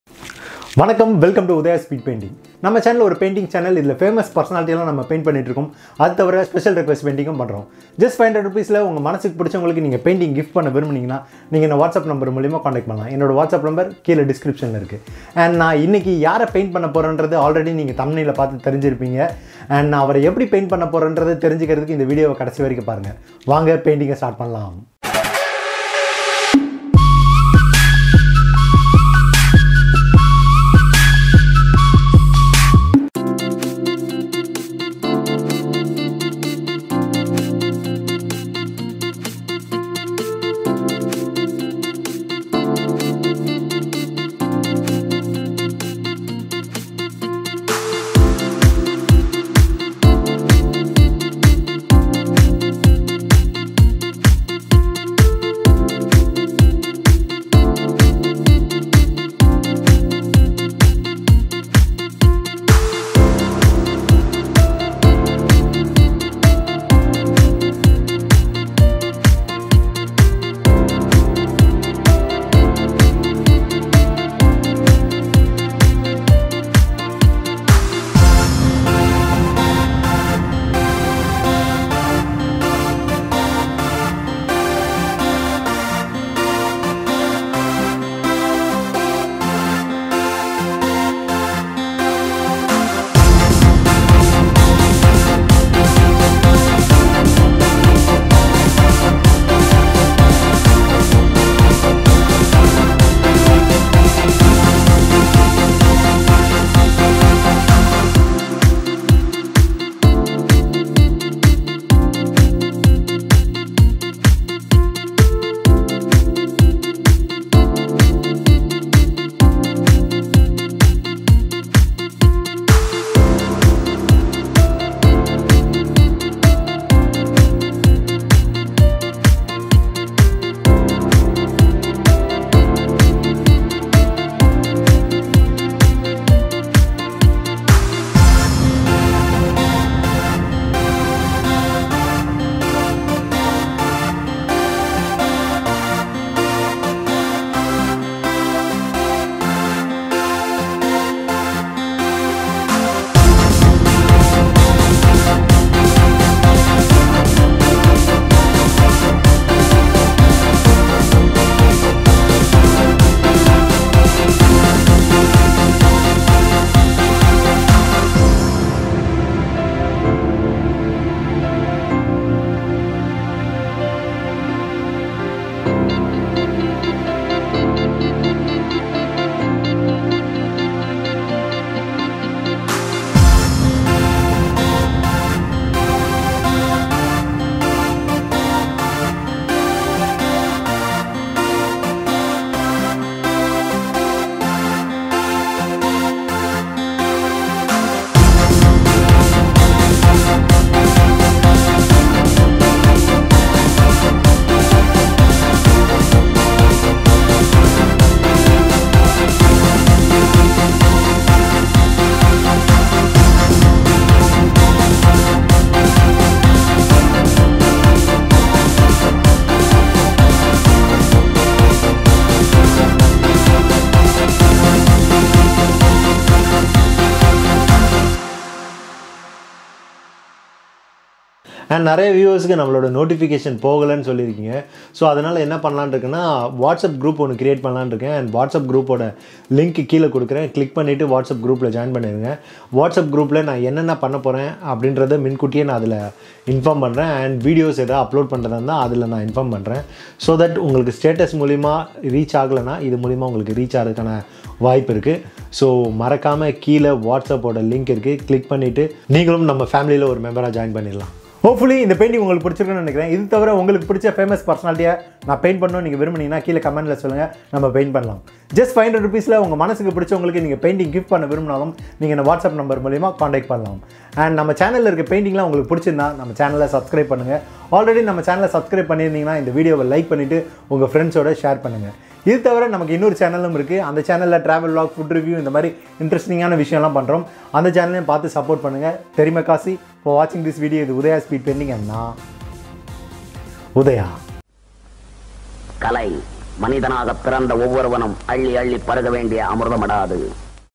How to Draw Mari Selvaraj Indian film director | Karnan | pariyerum perumal - Udhaya Speed Painting
Mari Selvaraj is a South Indian film director and author who works in the Tamil film industry. He made his directorial debut with Pariyerum Perumal, which opened to positive reviews and received many awards and accolades.